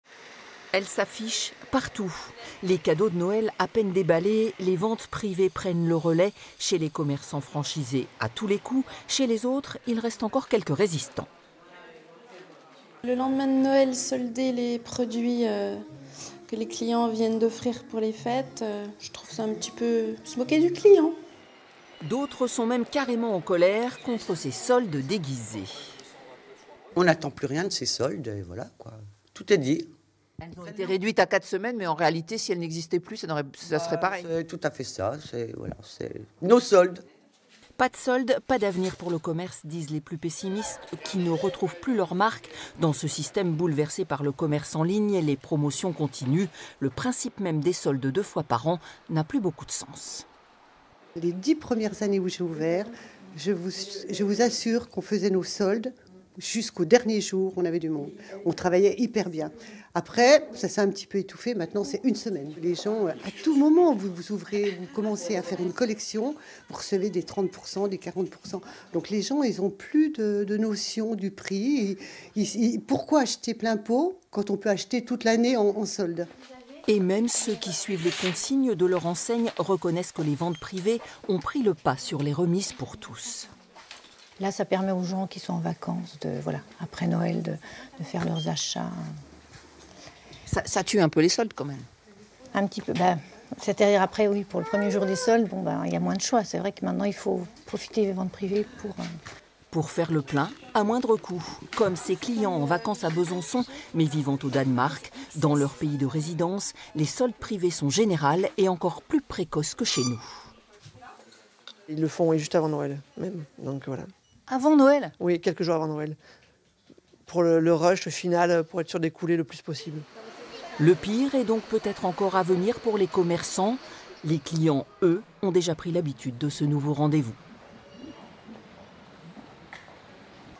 Besançon : Les ventes privées en attendant des soldes d'hiver raccourcis !
Reportage C.Schulbaum, S.Poirier
avec
Anne-Elaine Roland
Commerçante indépendante bijoux fantaisie
Julie Girard
Commerçante indépendante habillement
Françoise Jaillet
Commerçante indépendante chaussures
Françoise Greset
Commerçante franchisée habillement enfants
Christophe
Client venant de Copenhague (Danemark)